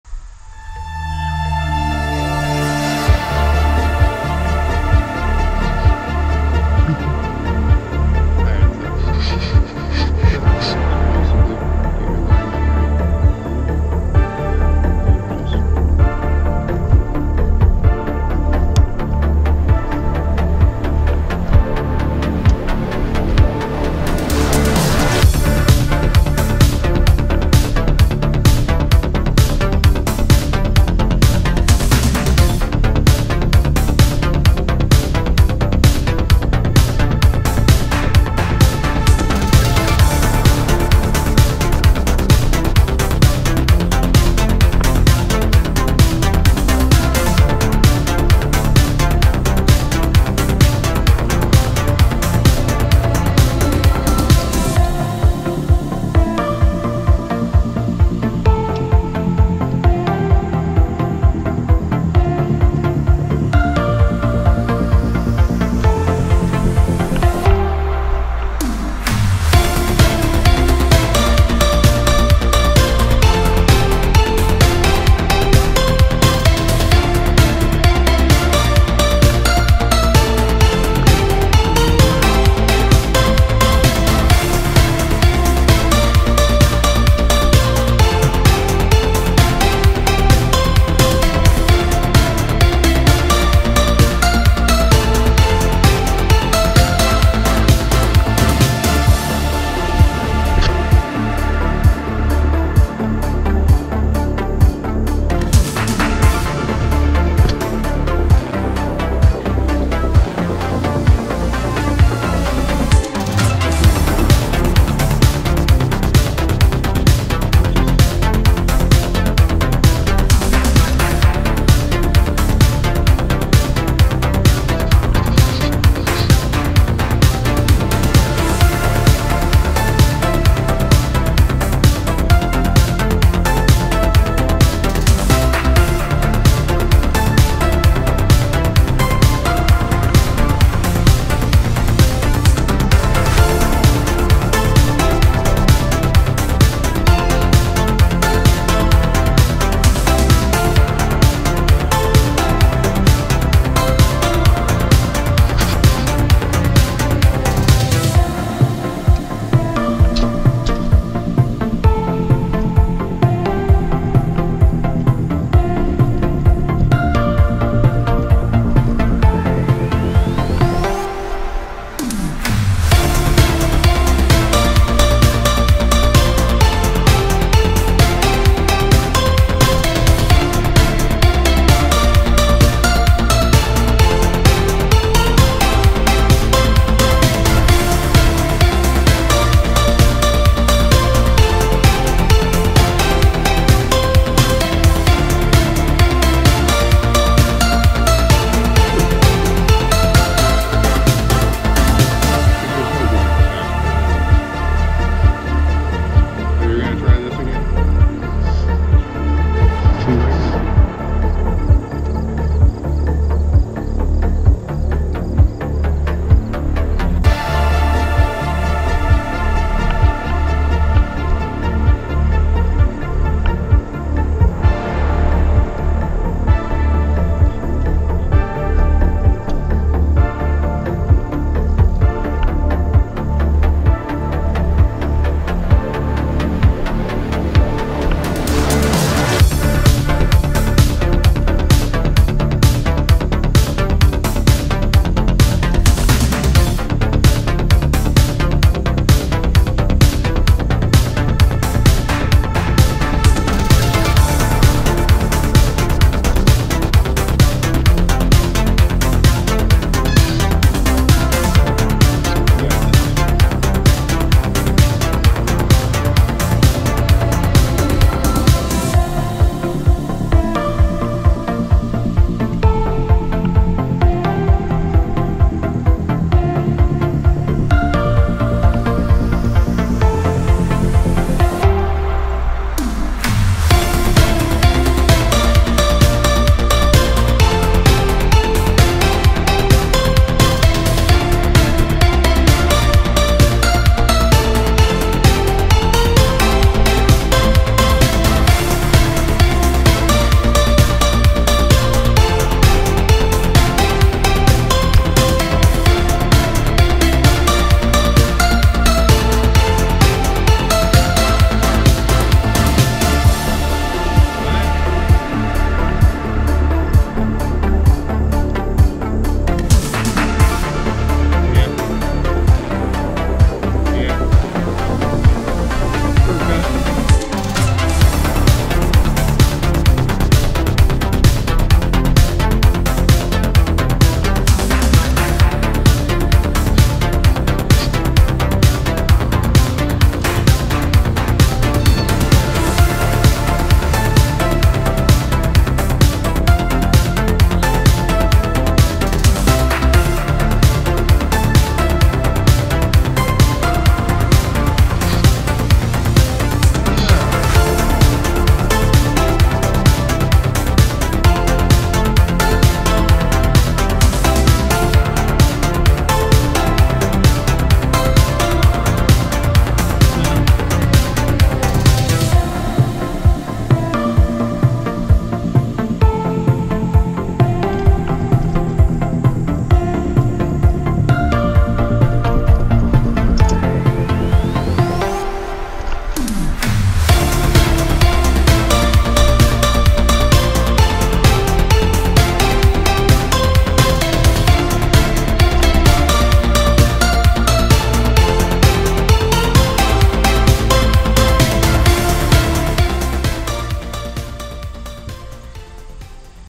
Back to Basics - Beginner Perspective on the Pump Track
Still teaching the in's and out's of riding a mountain bike, we travel once again to Austin Badger park to session the slalom track. Filled with tabletops, berms, and drops, this course is a fantastic way to learn your control basics or hone your skills!